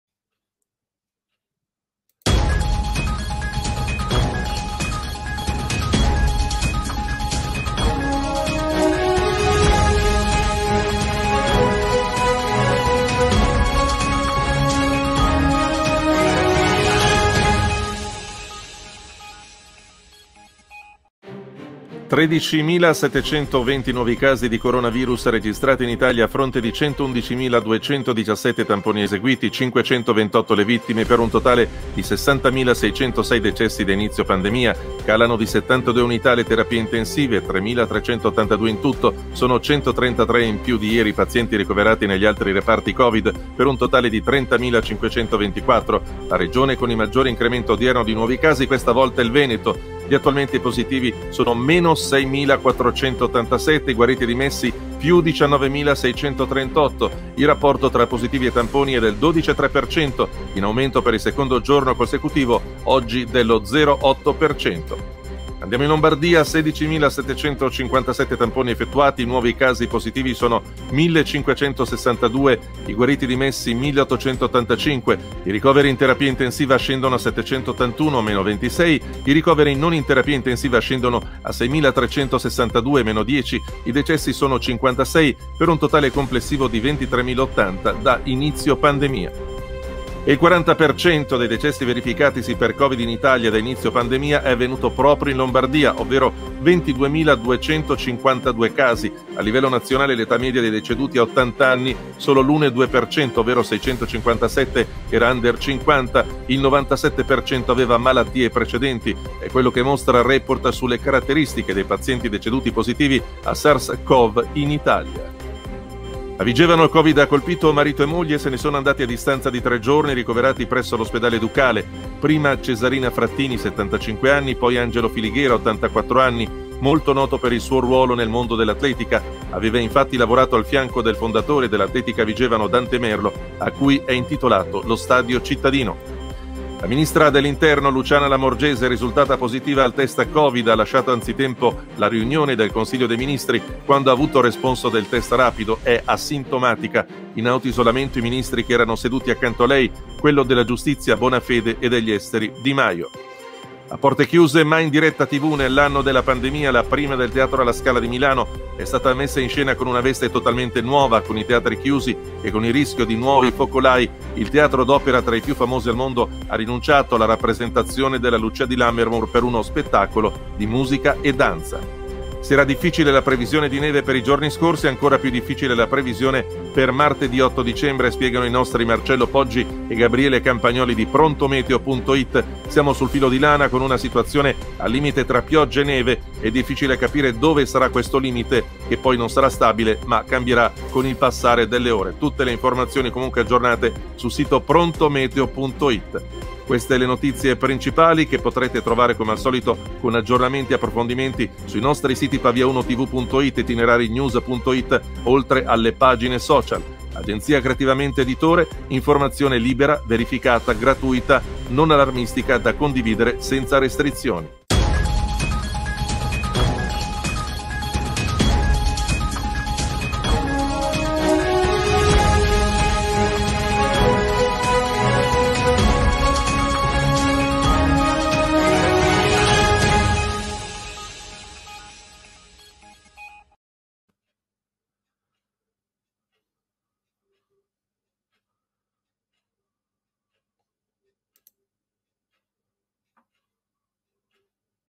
BREAKING NEWS - 7 DICEMBRE 2020
Le notizie del giorno in Lombardia e in provincia di Pavia in 3 minuti. Conduce: Diego Bianchi